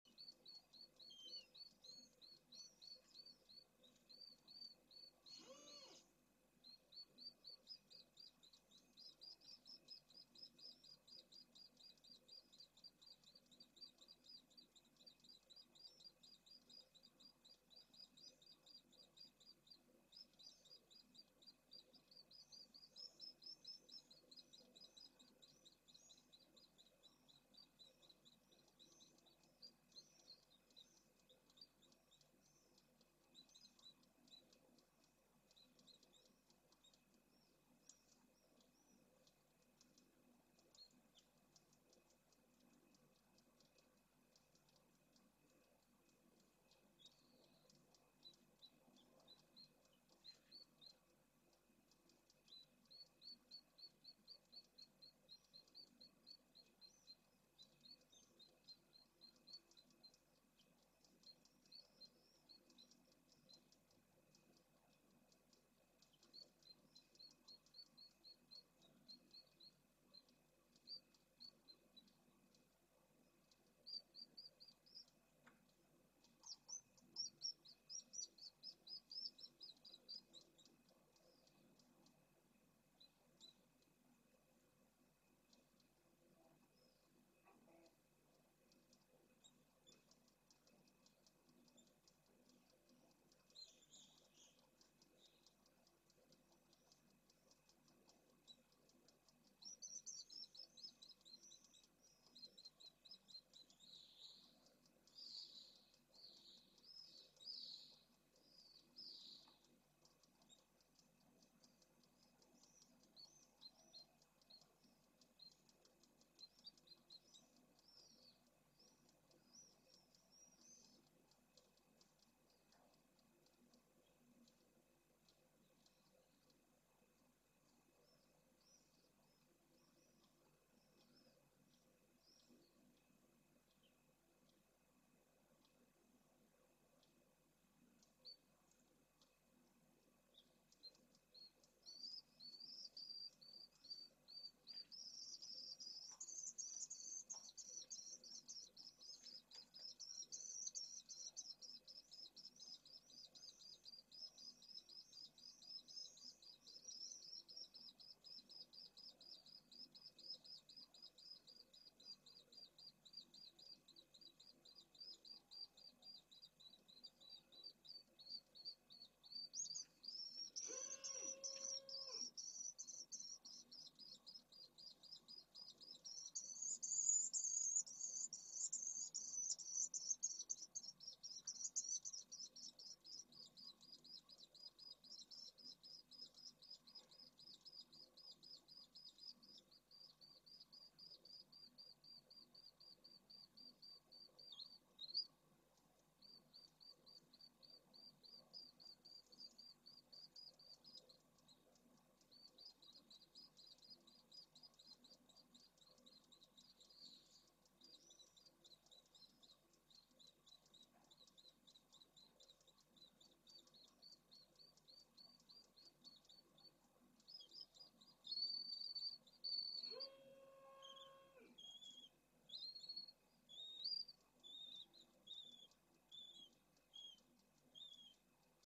2 HUEVOS EN N2 y pollos N1 y N3 - Vencejo pálido - Jerez de la Frontera, Esclavas de Jerez
Paseo virtual por todo el tambor, se puede ver que en el nido 2 ya hay dos huevos. A su lado se encuentra un progenitor.

Por otro lado, los pollos de los nidos 1 y 3 están muy desarrollados, siendo evidentes sus incipientes plumas de vuelo.

Cronología 2022:

27/05/22 - 🐣🐣
26/05/22 - 🐣

09/05/22 - 🥚🥚🥚
06/05/22 - 🥚🥚
04/05/22 - 🥚